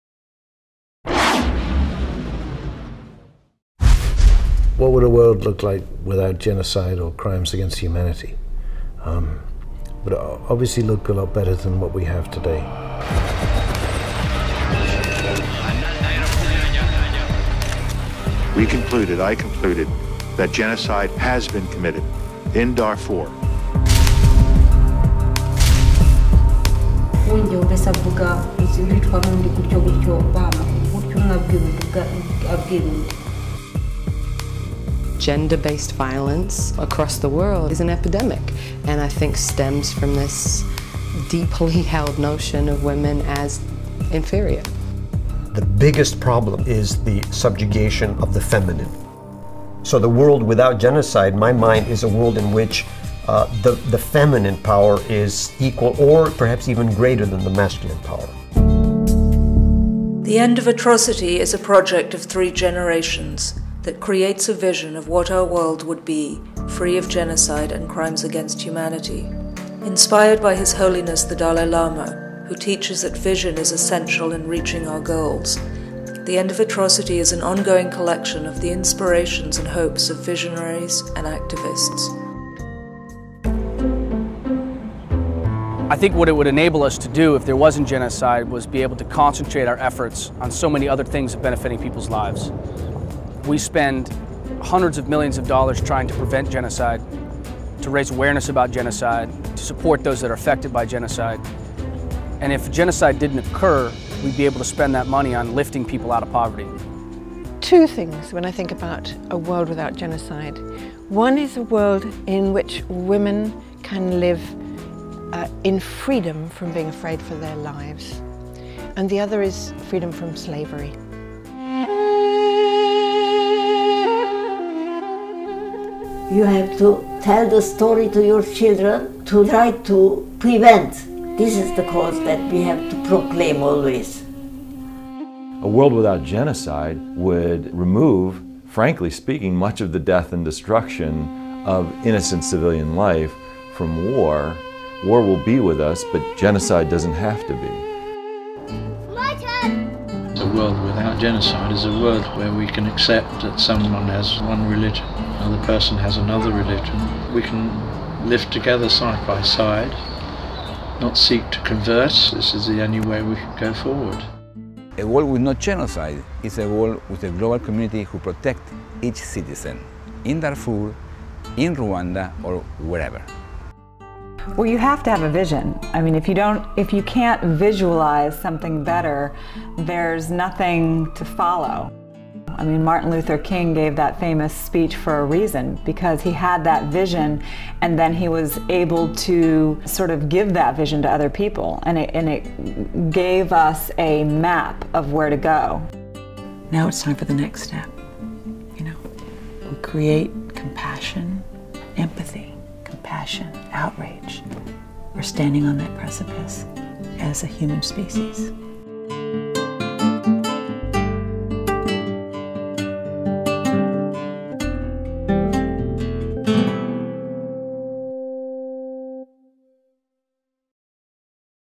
Imagine the End of Atrocity
If we don't have a vision of a better future can we really create one? Leaders as diverse as The Dalai Lama and Martin Luther King have shown us the need for vision to build a path to lasting peace and progress. Yet since the Holocaust and United Nations Universal Declaration of Human Rights in 1948, civil society has promised "never again" and still genocide and appalling atrocities have continued into the 21st century.We don't have a vision for how to build a world free from atrocity. We need one. Help us share these visions: become part of the movement: imagine the end of atrocity.